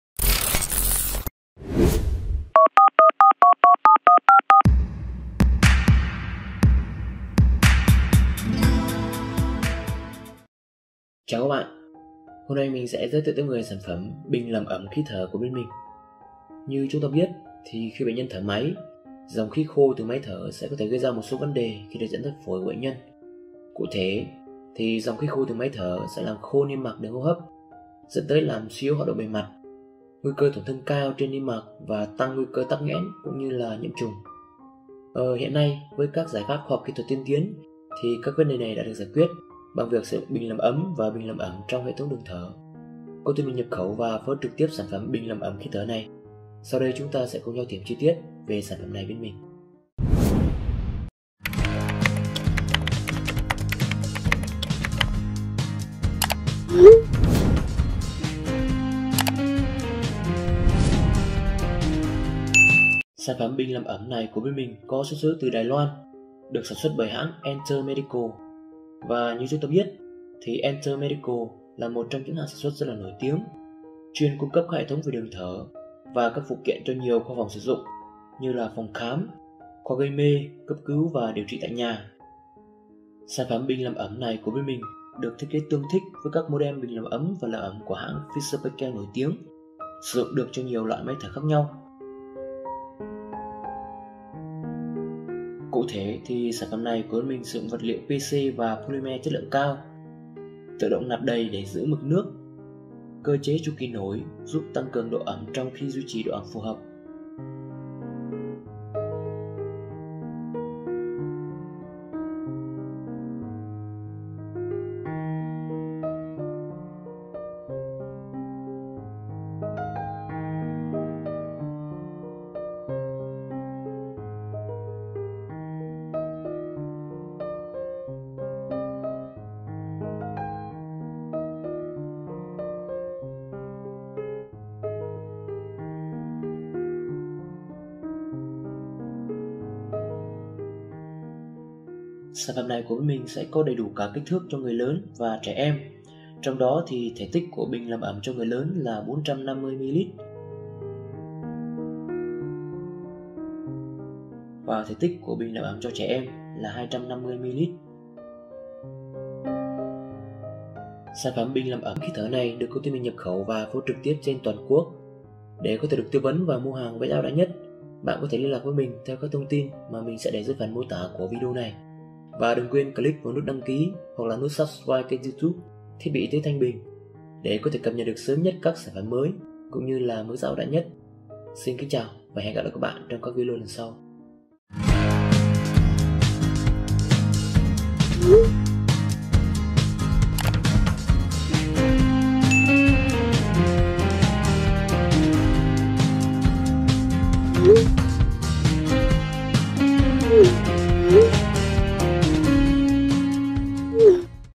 Bình làm ẩm khí thở | Humidifier Chamber
Khi bệnh nhân thở máy, dòng khí khô từ máy thở có thể gây ra vấn đề khi được dẫn đến phổi của bệnh nhân. Khí thở khô sẽ làm khô niêm mạc đường hô hấp, dẫn tới làm suy yếu hoạt động bề mặt, nguy cơ tổn thương cao trên niêm mạc và tăng nguy cơ tắc nghẽn, cũng như là nhiễm trùng.
Dựa trên các giải pháp kỹ thuật tiên tiến hiện nay, vấn đề này đã được giải quyết bằng việc sử dụng bình làm ấm và bình làm ẩm trong hệ thống đường thở
TẤT CẢ CÁC SẢN PHẨM CỦA CÔNG TY TNHH TBYT THANH BÌNH ĐỀU ĐƯỢC NHẬP KHẨU CHÍNH NGẠCH, CÓ ĐẦY ĐỦ TẤT CẢ CÁC LOẠI CHỨNG CHỈ & GIẤY TỜ
DANH SÁCH SẢN PHẨM CỦA CÔNG TY TNHH TBYT THANH BÌNH

♥ CẢM ƠN QUÝ KHÁCH HÀNG ♥